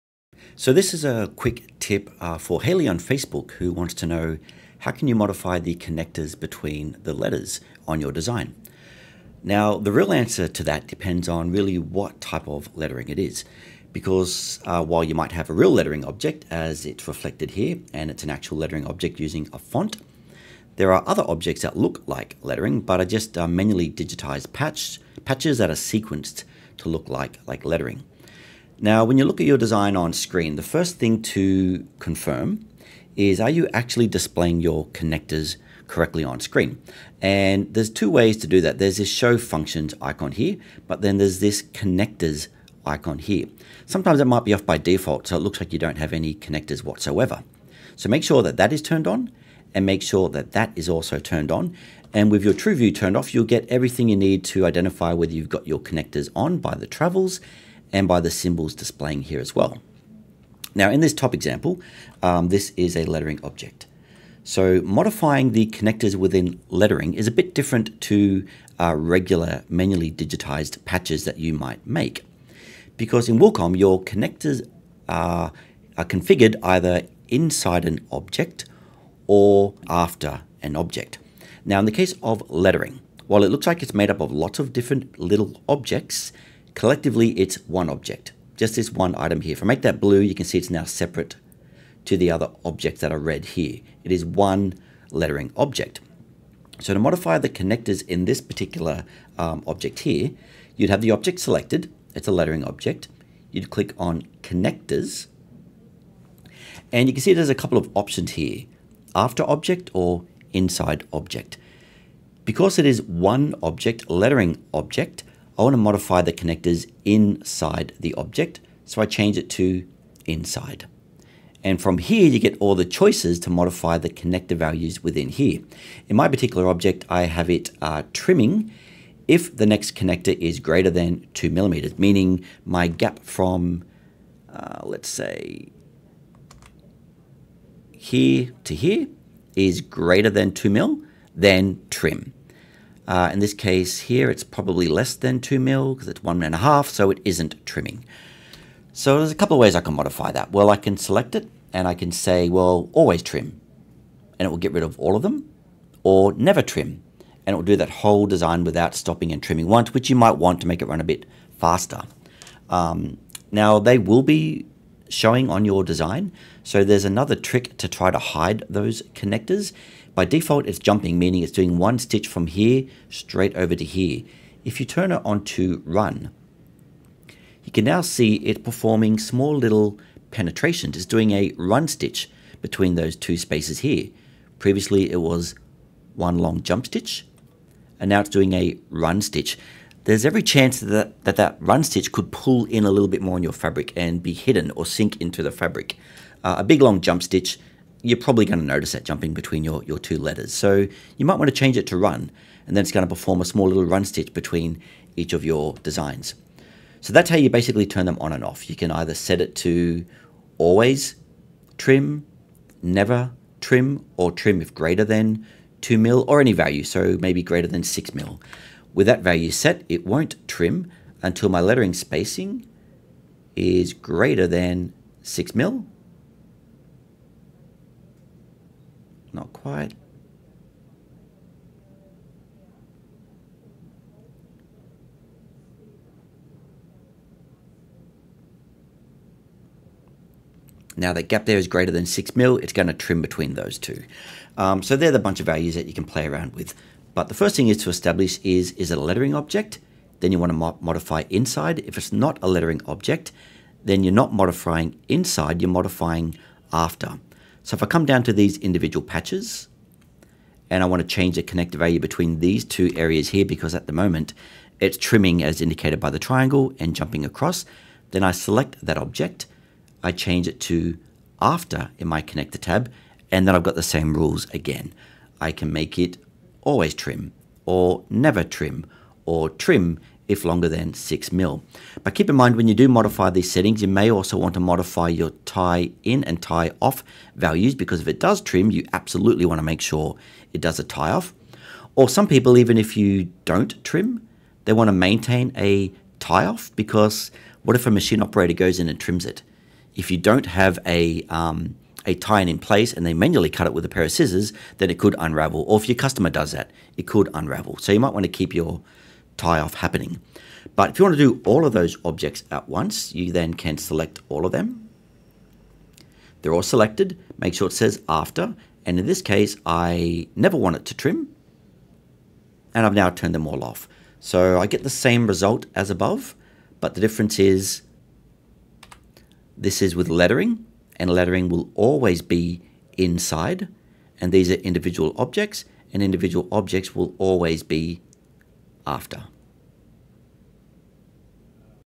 How to modify connectors between embroidery lettering - Wilcom Quick Tip
Brenden answers a Facebook question with a Quick Tip on how to modify connectors between letters in Wilcom EmbroideryStudio.